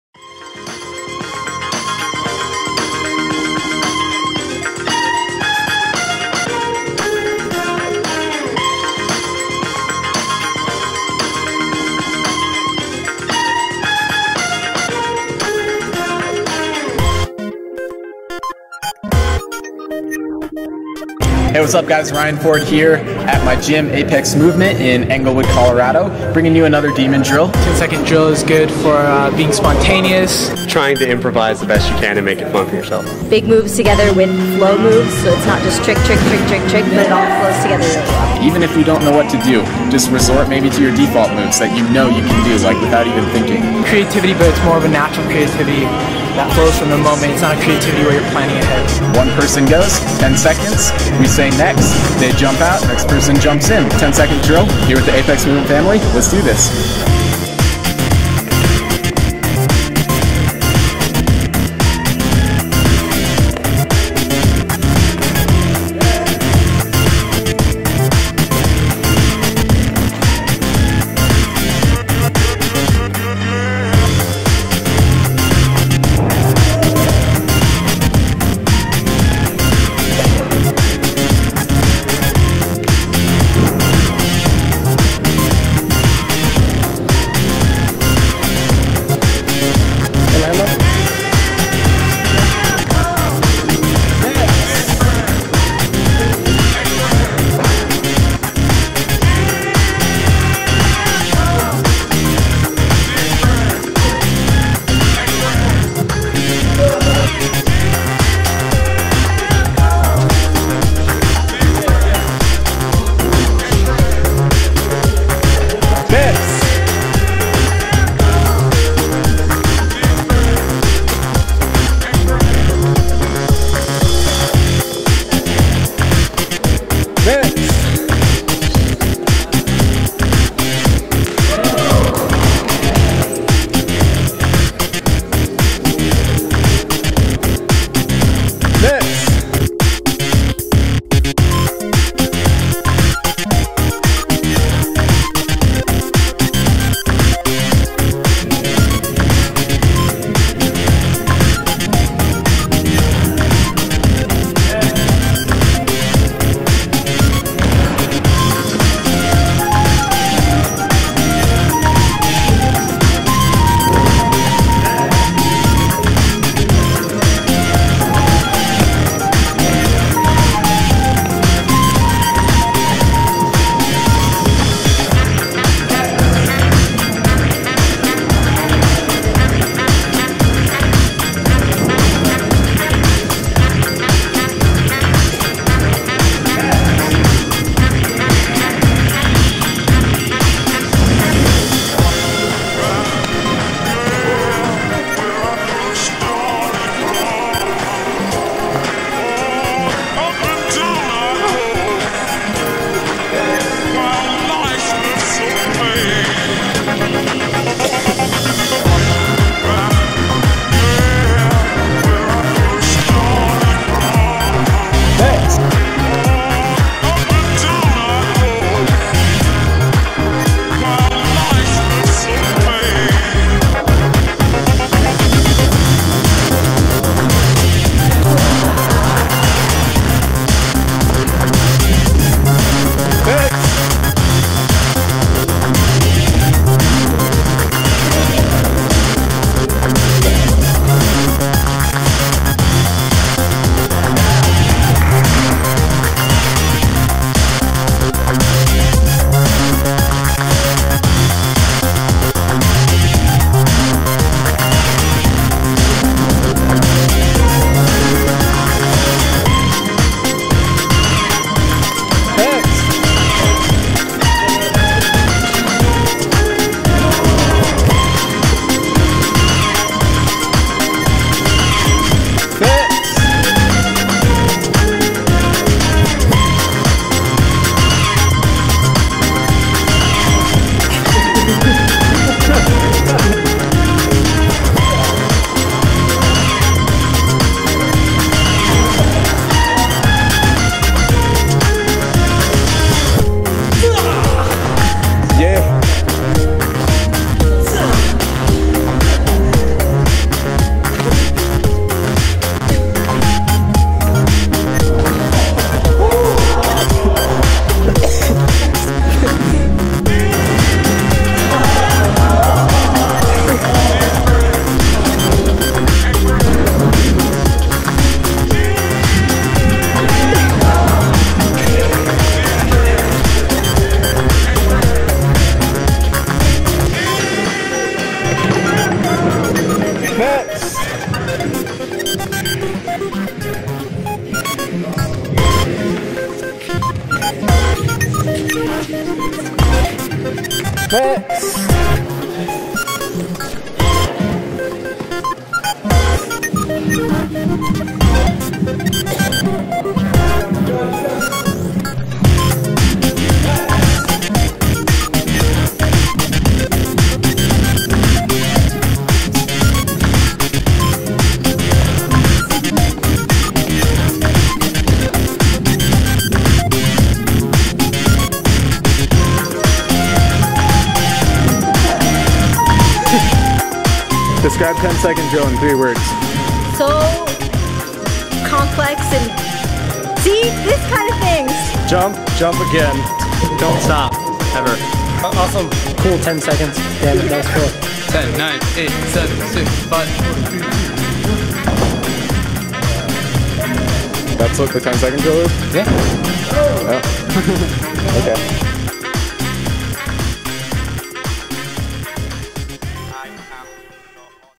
10 Second Drill - Parkour Training
The purpose of this drill is to improve your spontaneous, thoughtless flow by moving creatively through a small, compact set of obstacles for 10 seconds at a time. By not planning your movement beforehand, you will be challenged to creatively improvise, react, and adapt in the moment.

Stay up to date with my latest projects!